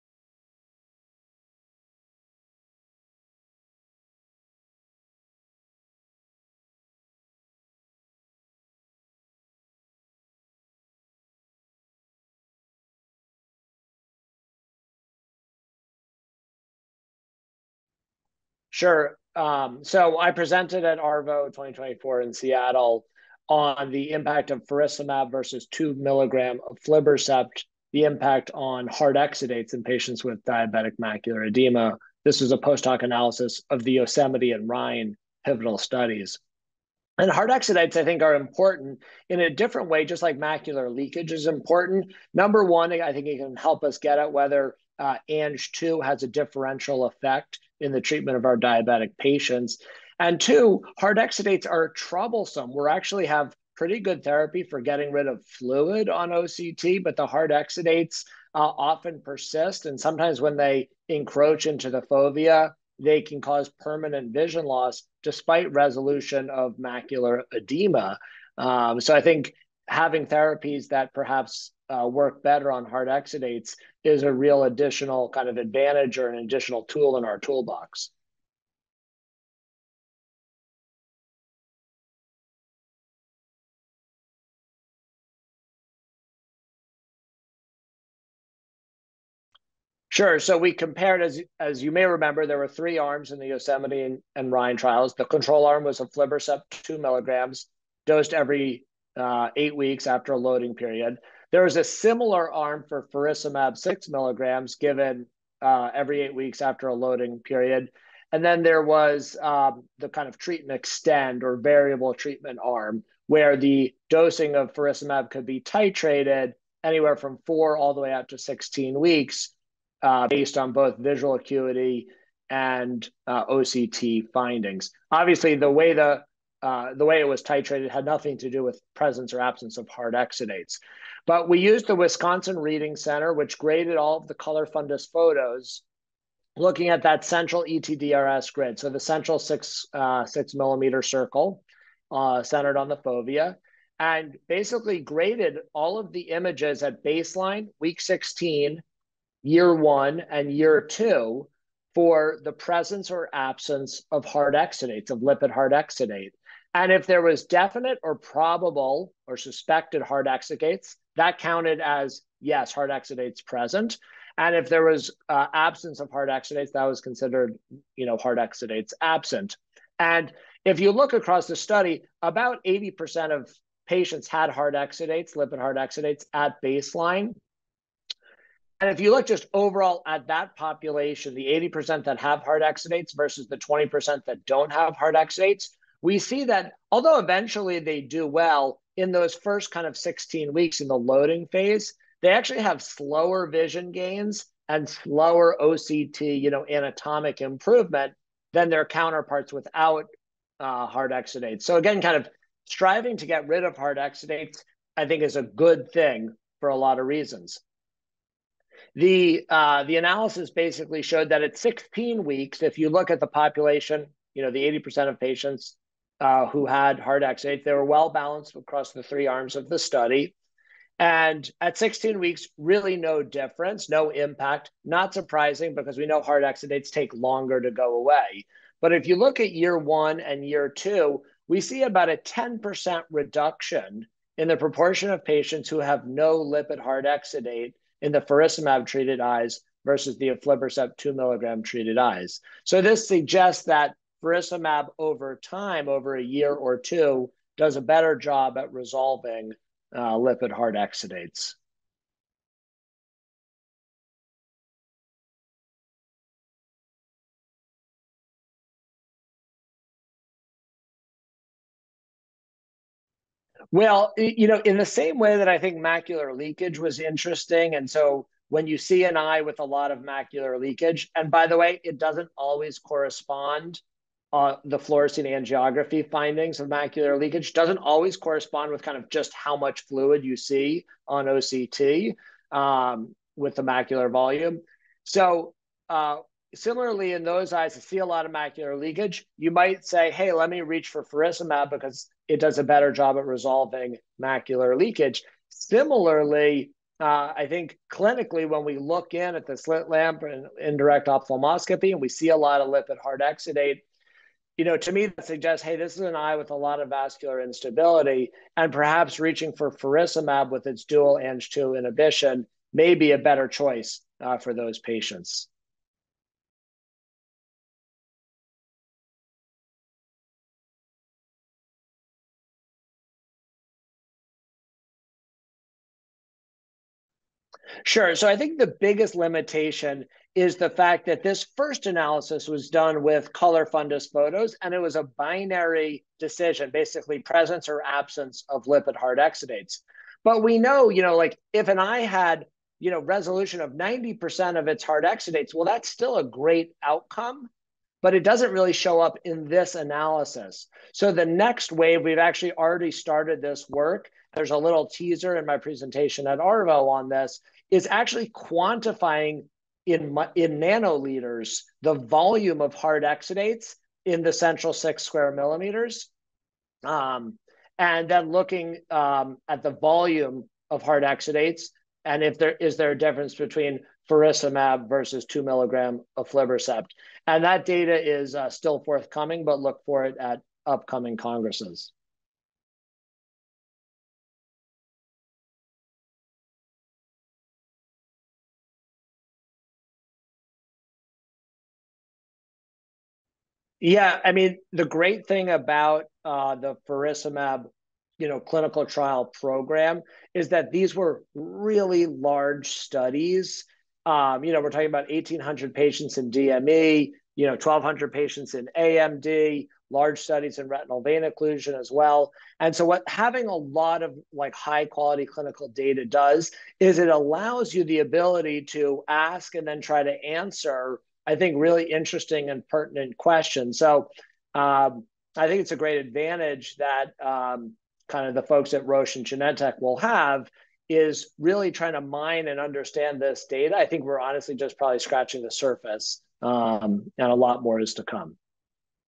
Roger A. Goldberg, MD: Impact of Dual Inhibition on Hard Exudates in DME
Dual angiopoietin-2/vascular endothelial growth factor (VEGF)-A inhibition with faricimab achieved numerically greater reductions in hard exudates than aflibercept in eyes with diabetic macular edema (DME), according to an exploratory analysis presented at the 2024 Association for Research in Vision and Ophthalmology (ARVO) Meeting.

In an interview with HCPLive, investigator Roger A. Goldberg, MD, a vitreoretinal surgeon at Bay Area Retina Associates, described how patients with hard exudates exhibited no difference between the treatment arms after the loading phase. However, those treated with faricimab saw a notable reduction in hard exudates at the 52-week and 96-week marks.

“If you look at year 1 and year 2, we see about a 10% reduction in the portion of patients who have no lipid hard exudates in the faricimab-treated eyes versus the aflibercept 2 mg-treated eyes,” Goldberg told HCPLive. “This suggests that faricimab, over time, does a better job at resolving hard exudates.”

Highlights
0:07 Importance of reducing hard exudates in DME
1:20 Impact of dual inhibition on hard exudates
4:13 Effect on clinical judgment in DME
5:18 Limitations of analysis
6:23 Future analyses